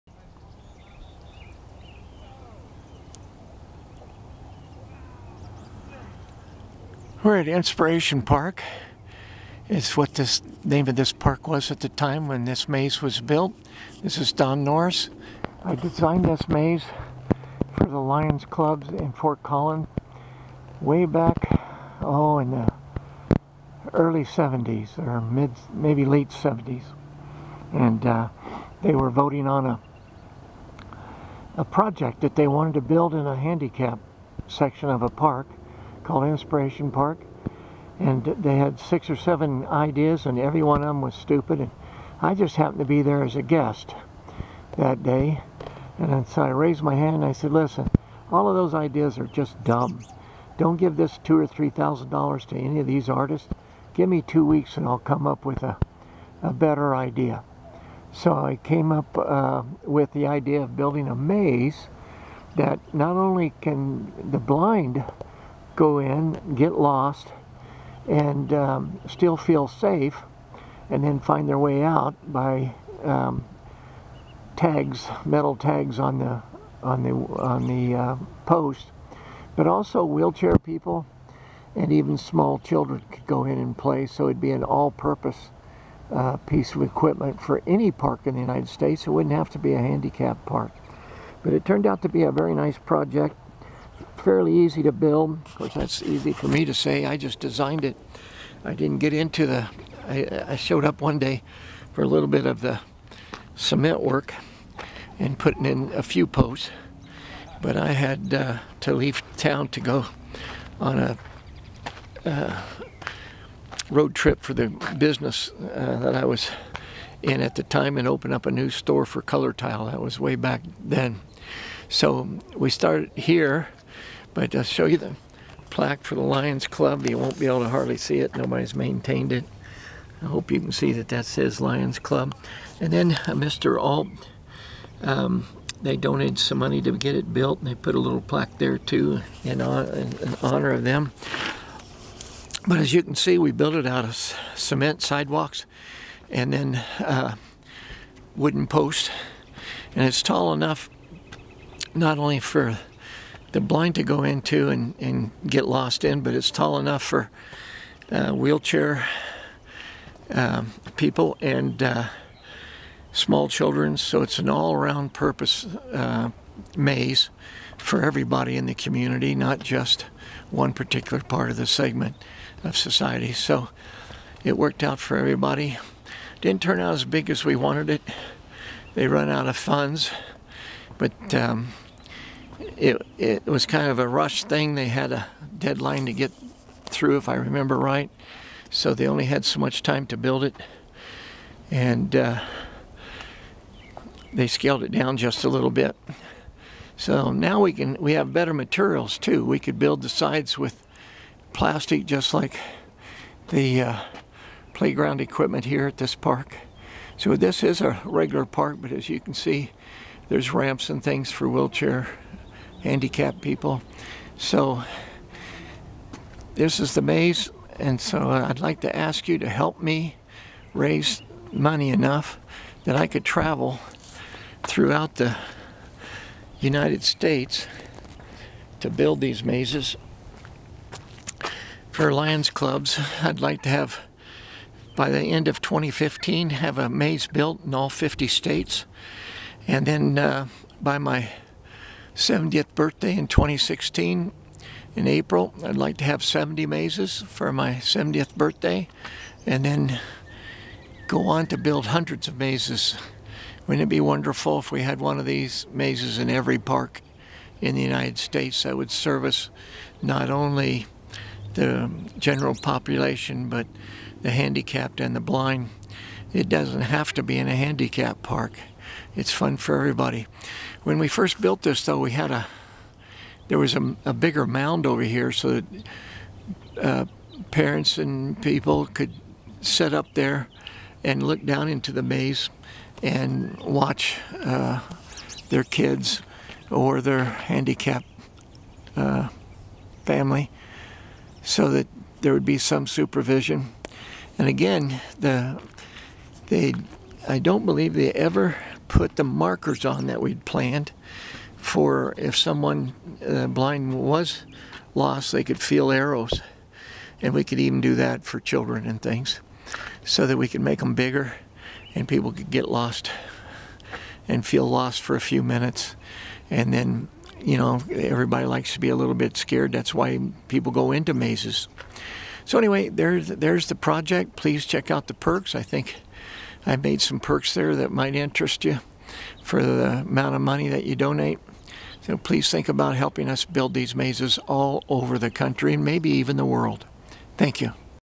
Maxe 013
I feel that I "invented" the "Maze for the Blind and the Mind", a maze that could be built in any park and not just for the handicapped visitors, to be used by all visitors. I came up with the idea, then designed this maze in Ft Collins, Colorado, was only able to help a little with the  building of it, and it has been on my mind for over 30 years. It is hard to express the satisfaction of seeing it still being used after more than 30 years. Please check my project on StartSomeGood.com.